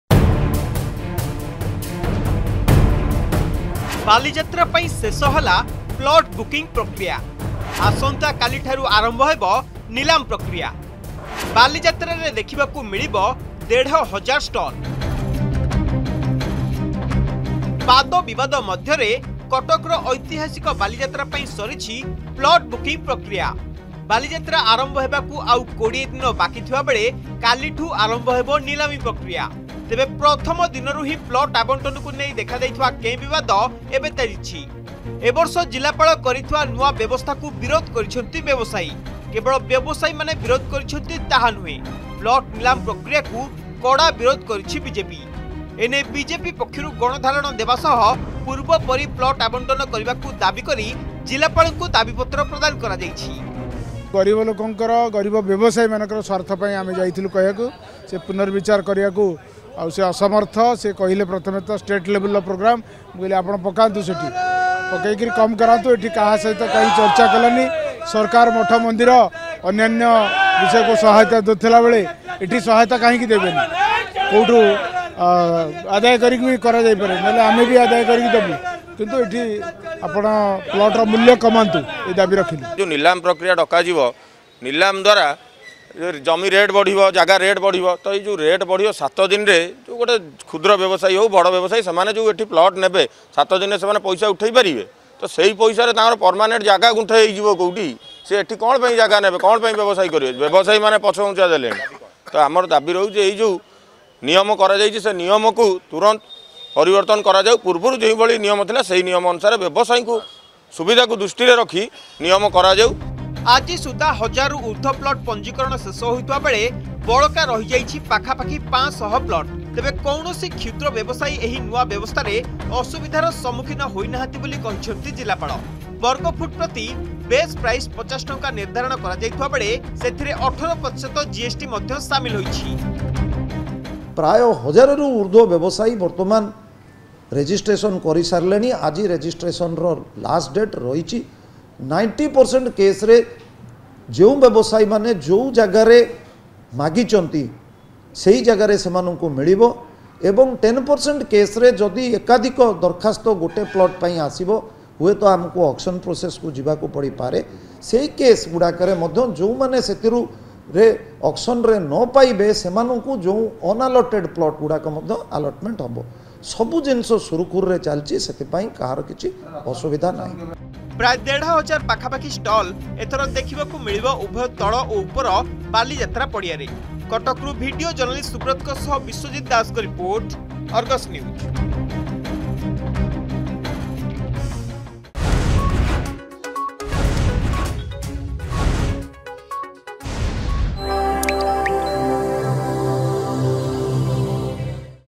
ବାଲିଯାତ୍ରା ପାଇଁ ଶେଷ ହେଲା ପ୍ଲଟ ବୁକିଂ ପ୍ରକ୍ରିୟା
ଆସନ୍ତାକାଲିଠାରୁ ଆରମ୍ଭ ହେବ ନିଲାମ ପ୍ରକ୍ରିୟା
ପ୍ରାୟ ଦେଢ଼ ହଜାର ପାଖାପାଖି ଷ୍ଟଲ ଏଥର ଦେଖିବାକୁ ମିଳିବ ଉଭୟ ତଳ ଓ ଉପର ବାଲିଯାତ୍ରା ପଡ଼ିଆରେ । ଏଥର ନୂଆ ଢାଞ୍ଚାରେ ନିଲାମ ପକ୍ରିୟା କରି ପ୍ରଶାସନ ବ୍ୟବସାୟୀଙ୍କୁ ପ୍ଲଟ ଆବଣ୍ଟନ କରିଛି । ତେବେ ବିଭିନ୍ନ ମହଲରୁ ଏହାକୁ ବିରୋଧ କରାଯାଇଛି ।